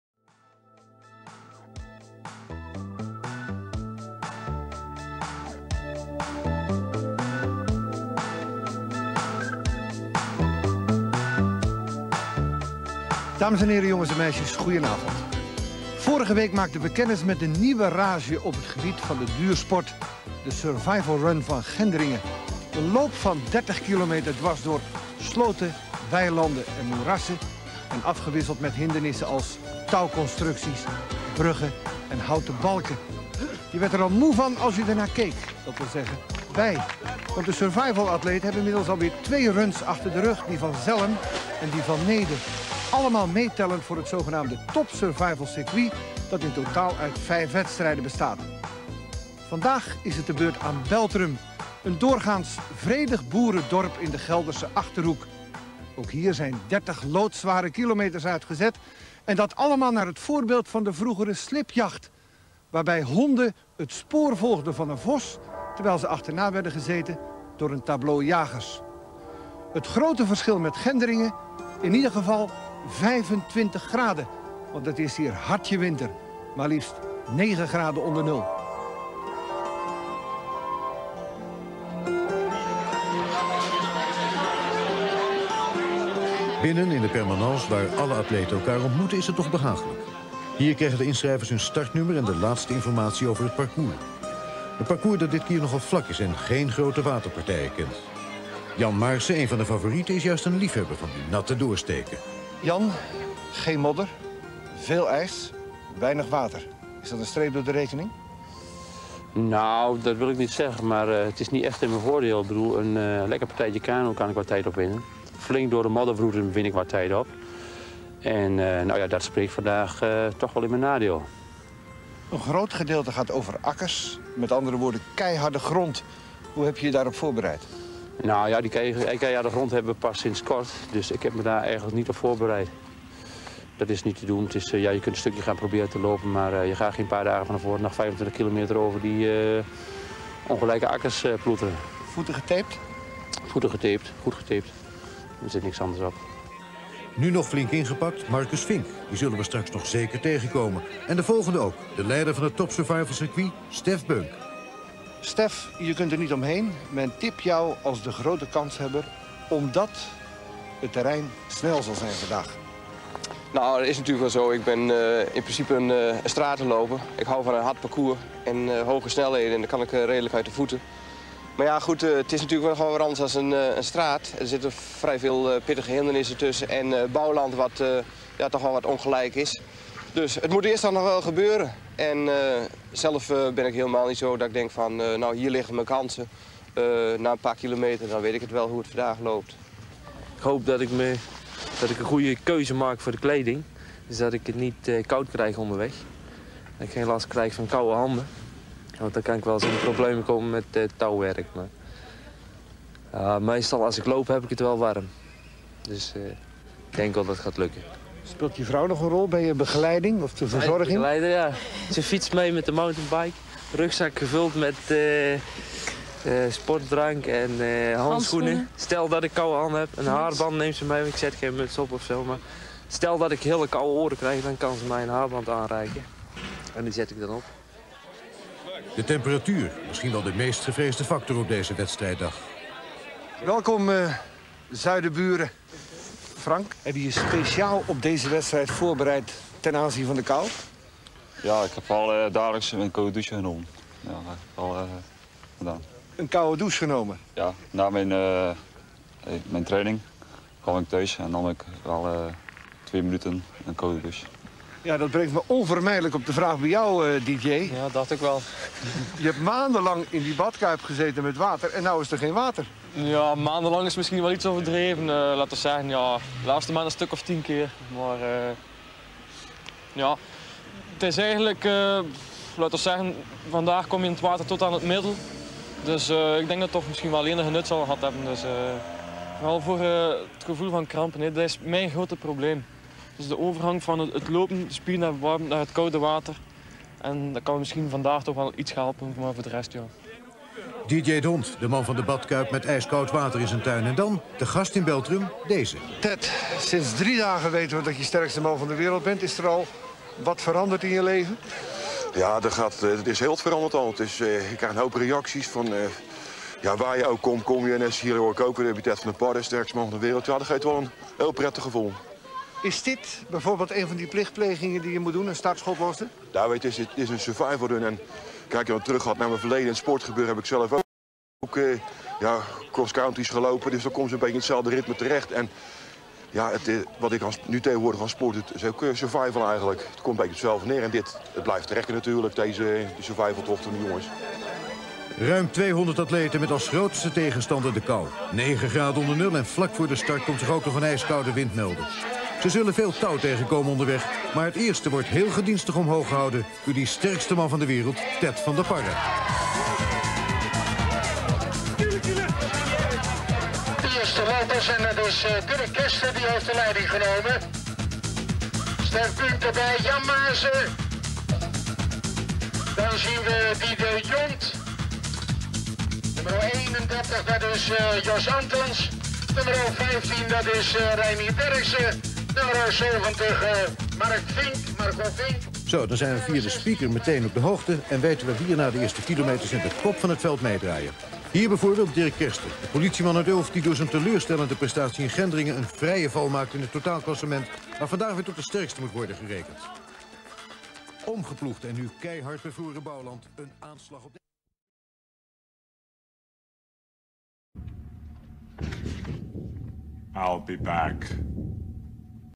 1992 Survival run Beltrun startschot door Ted van der Parre (net 3 dagen sterkste man van de wereld)
1992 Survival run Beltrun met het startschot door Ted van der Parre (net 3 dagen sterkste man van de wereld)